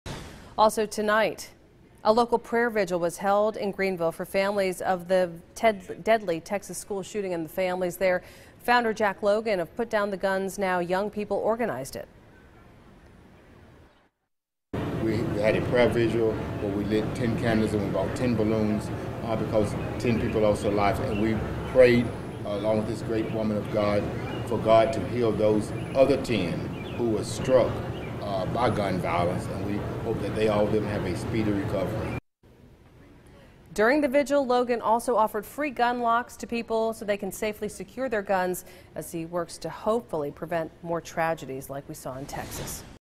Local vigil held for victims of Friday's school shooting in Texas
A vigil was held in downtown Greenville, Friday night, to remember the victims of the deadly school shooting in Texas.